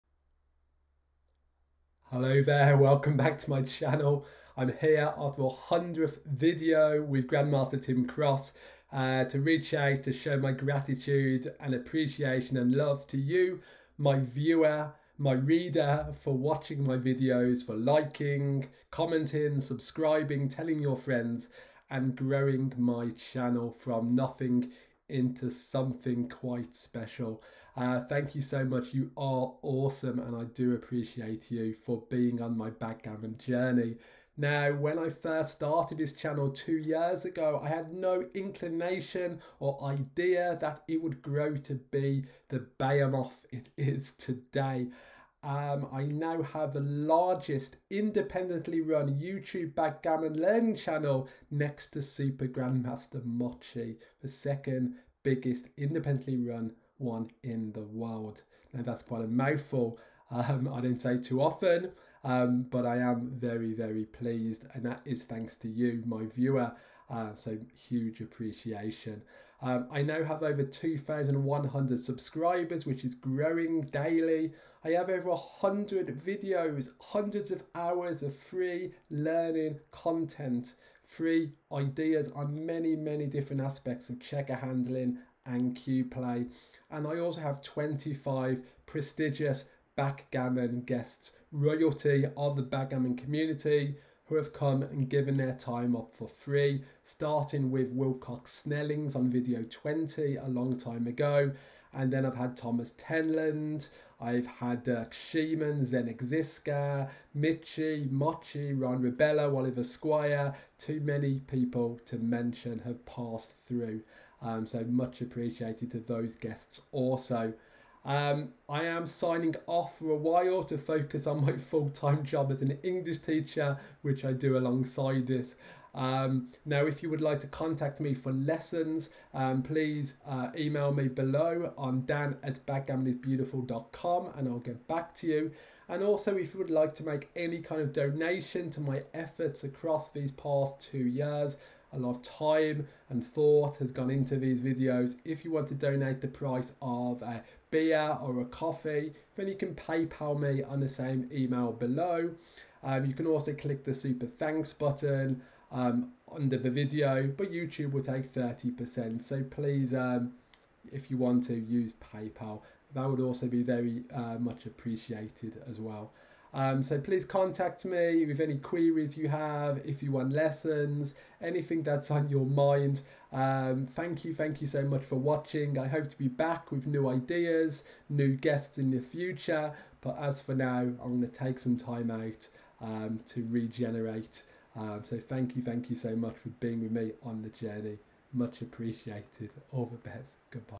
Backgammon: Thank You!
Thank you my viewer for being with me on my backgammon journey these past two years. It's now time for a break to re-energise and come back with more content.